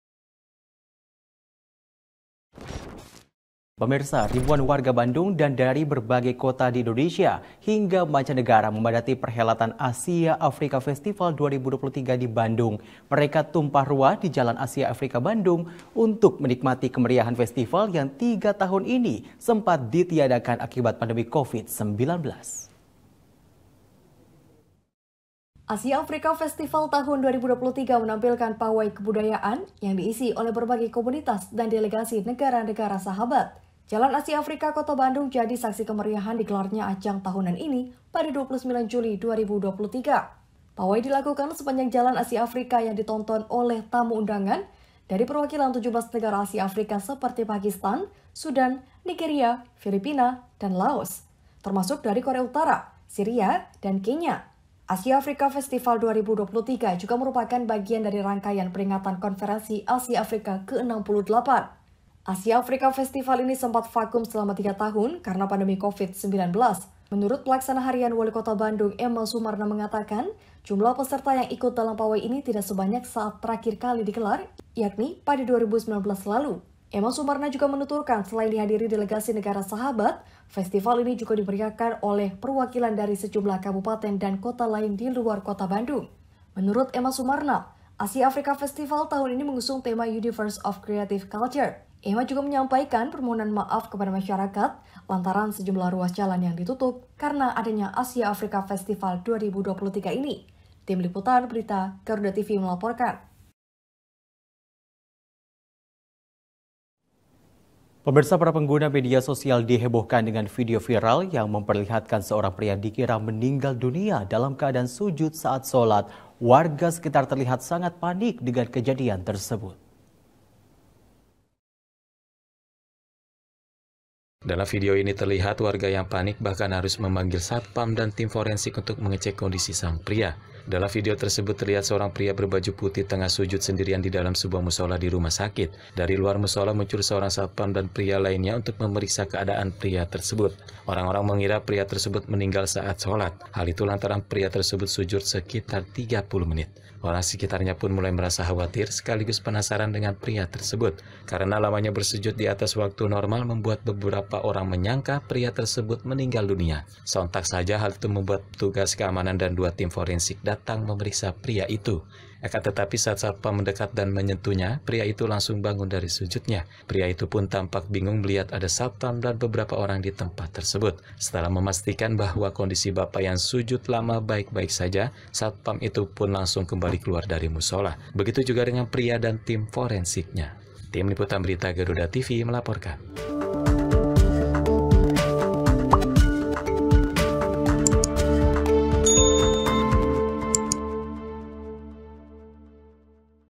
Asia Africa Festival 2023 di Bandung Memikat Ribuan Pengunjung
Jalan Asia Afrika Kota Bandung berubah menjadi panggung megah yang menyaksikan kemeriahan Asia Afrika Festival pada tanggal 29 Juli 2023. Ajang tahunan ini memperlihatkan parade kebudayaan yang diisi oleh berbagai komunitas dan delegasi dari negara-negara sahabat.

Parade kebudayaan berlangsung sepanjang Jalan Asia Afrika dan disaksikan oleh tamu undangan dari perwakilan 17 negara Asia Afrika, termasuk Pakistan, Sudan, Nigeria, Filipina, Laos, dan bahkan dari negara-negara jauh seperti Korea Utara, Suriah, dan Kenya. Asia Afrika Festival 2023 juga menjadi bagian dari rangkaian peringatan Konferensi Asia Afrika ke-68.

Acara ini merupakan penyelenggaraan ulang setelah mengalami tiga tahun vakum akibat pandemi COVID-19. Menurut pelaksana harian Walikota Bandung, Ema Sumarna, jumlah peserta pada parade kali ini tidak sebanyak saat terakhir kali digelar pada tahun 2019. Namun, semangat dalam mempersembahkan kebudayaan dari berbagai belahan dunia tetap menyala, mengukuhkan persahabatan dan kerja sama antara negara-negara Asia dan Afrika.